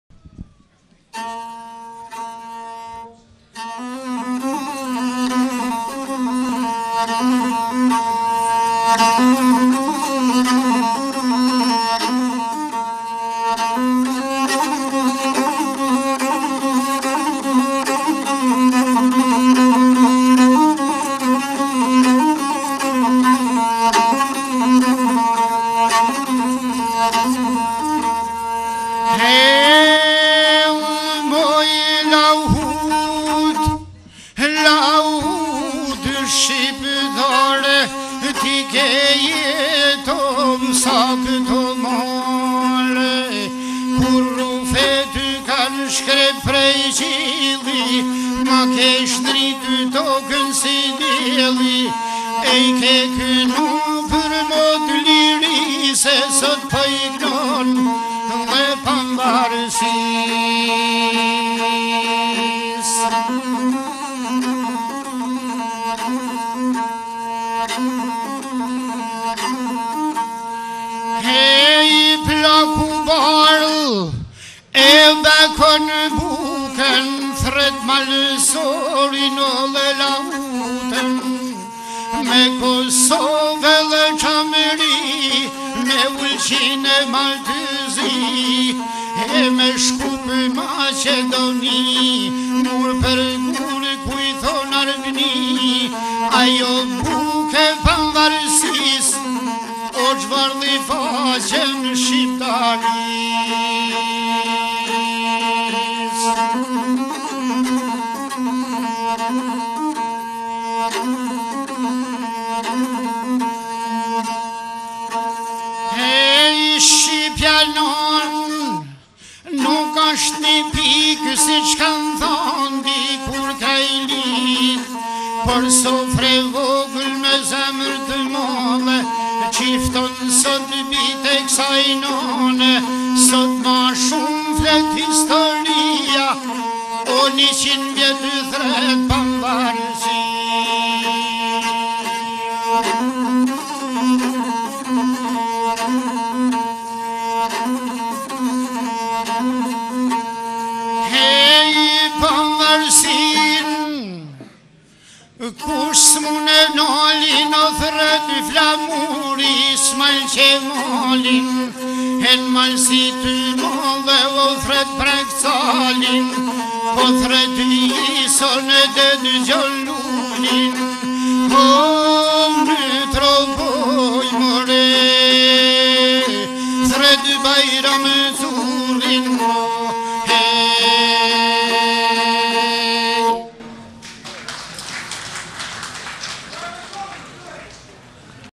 Televizioni Lezha PJETER MATUSHA KENGE PER PAVARESINE
Pjese nga Festivali Folklorik Tipologjik Kombetar 2013 Lezhe